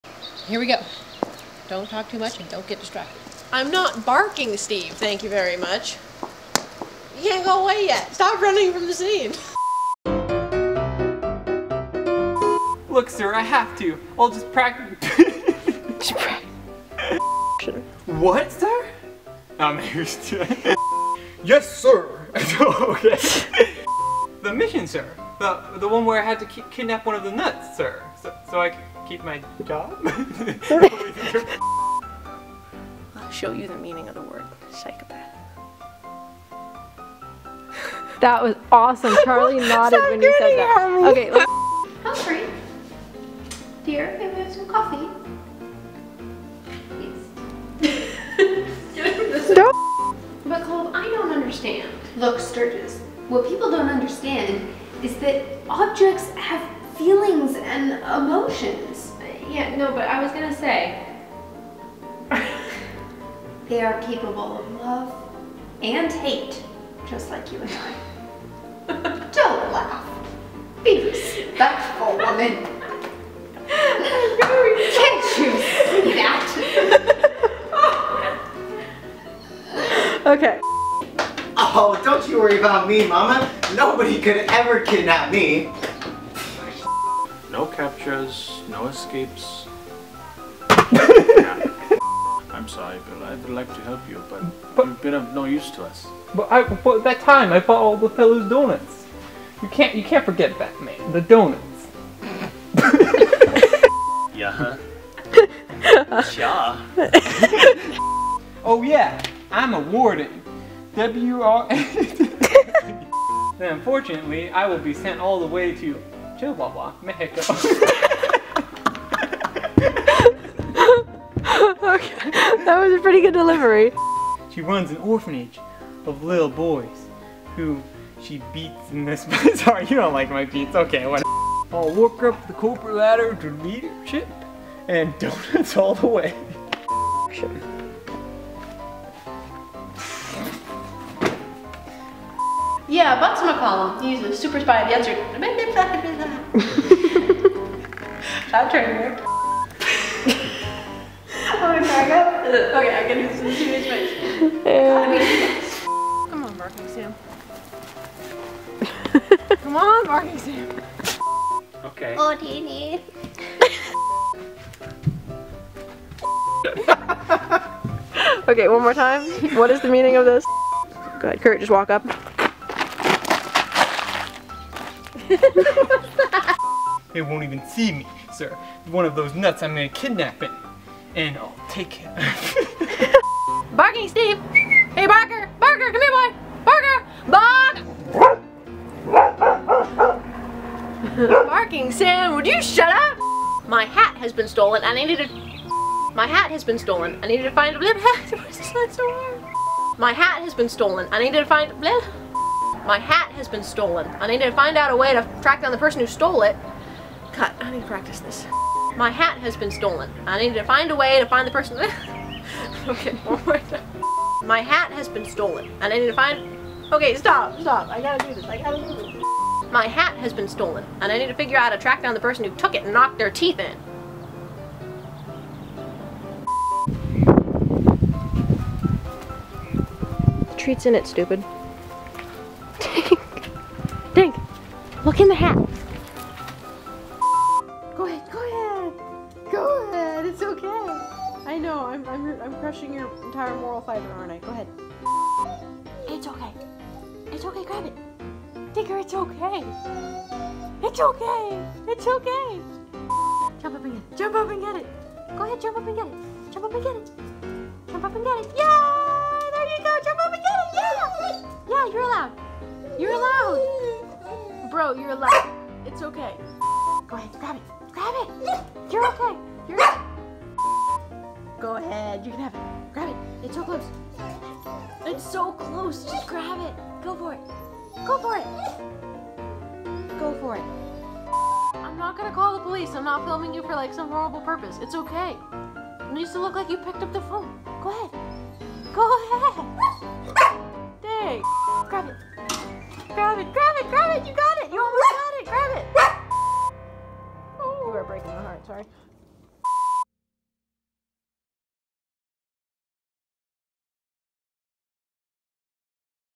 Parry & the Plattler Gag Reel
The official gag reel and outtakes of Episode 5: Parry & the Plattler.